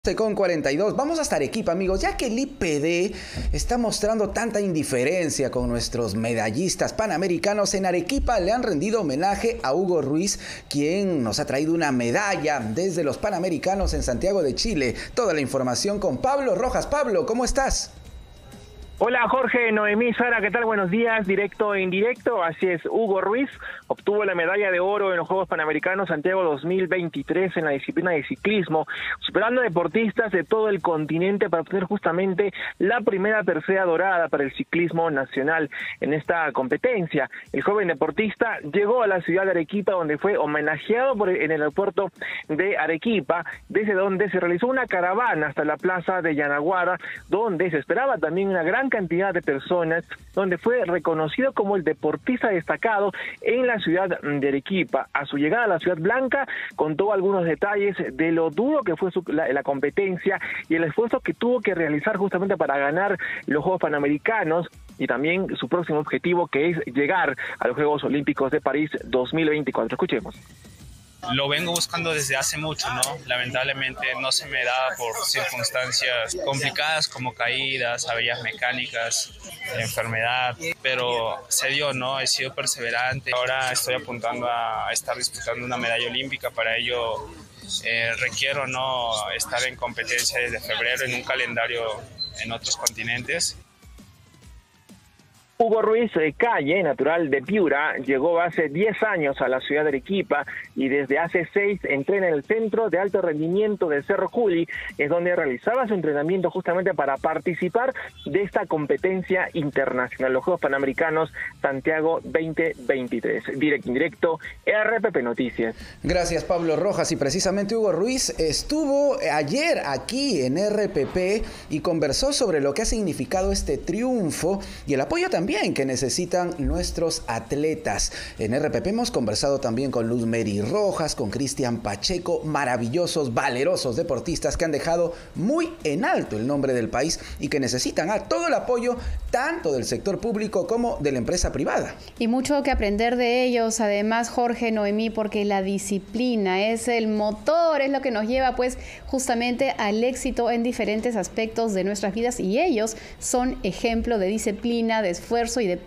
Arequipa: Realizan homenaje al medallista de oro Hugo Ruiz
🥇 Hugo Ruiz, el medallista de oro en los Juegos Panamericanos Santiago 2023, fue recibido con un homenaje en Arequipa por su triunfo en la disciplina de ciclismo.